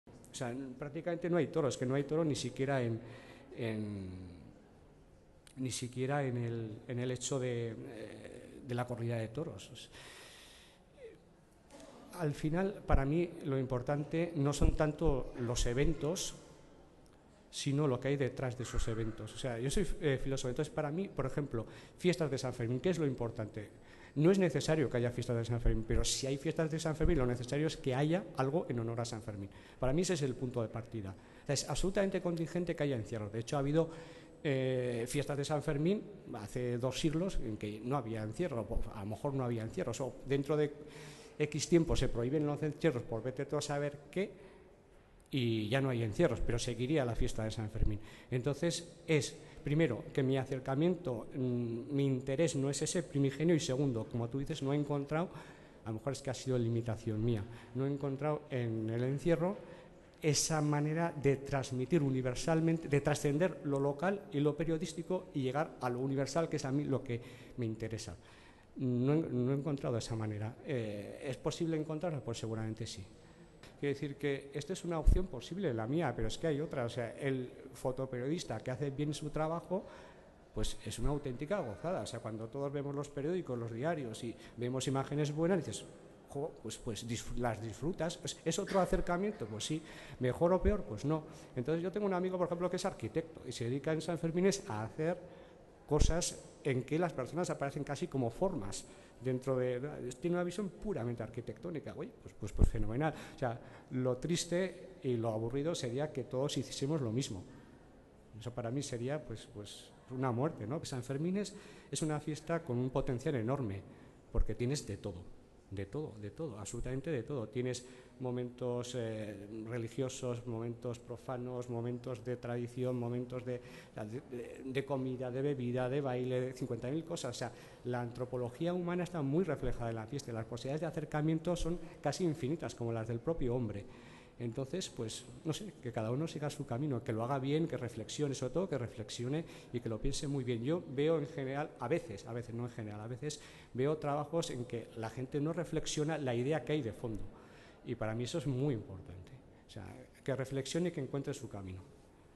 Exposición de fotografías de Sanfermin de Guillermo Navarro Úbeda
El fotógrafo y filosofo Guillermo Navarro espone en el Palacio del Condestable de Pamplona sus mejores imágenes de Sanfermin.